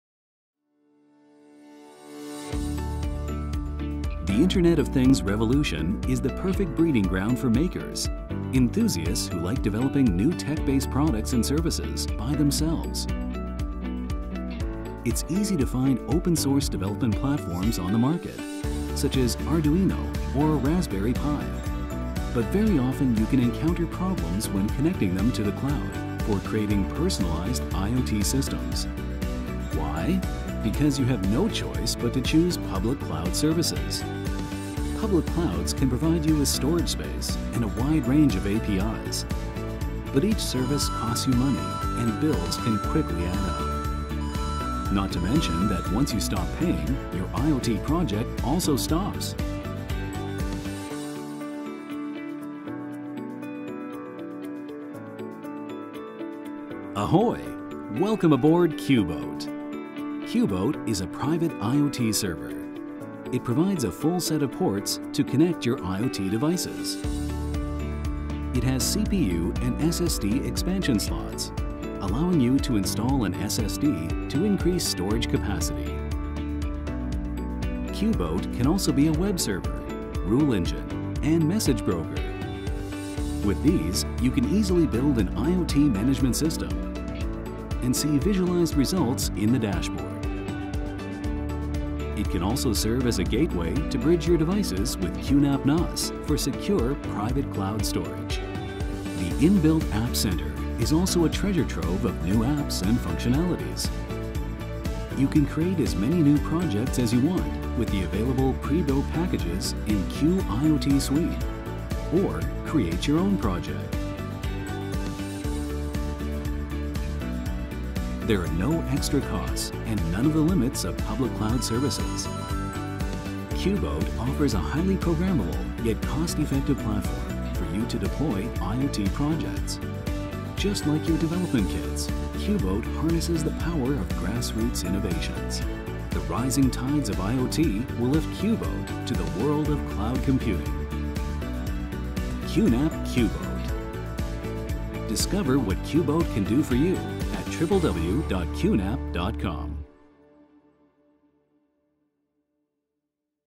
QNAP QBoat - Privater IoT-Server (DE)
QBoat bietet eine hochgradig programmierbare und dennoch kosteneffektive Plattform, damit Sie IoT-Projekte bereitstellen können. Die steigende Flut von IoT befördert QBoat in die Welt des Cloud Computing.